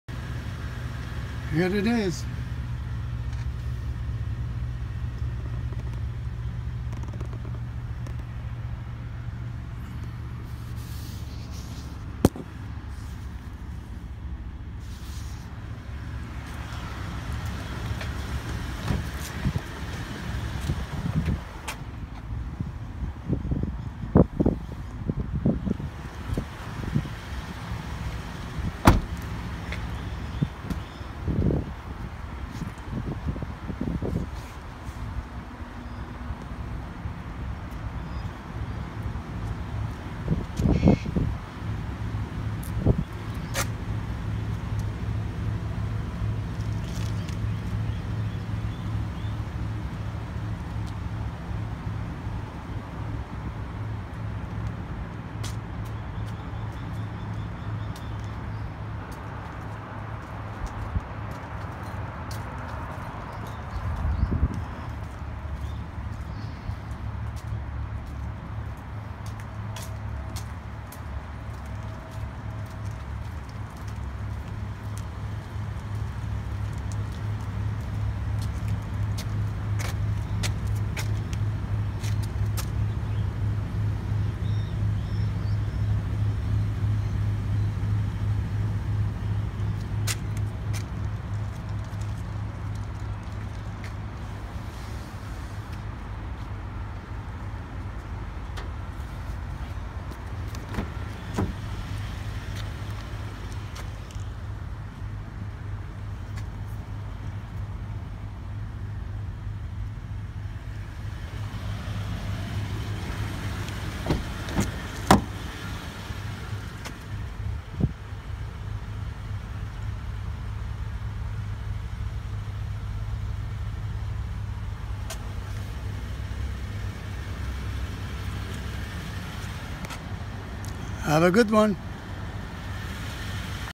Air brake for pick up.
Here is the air brake installed on the Tundra pick up.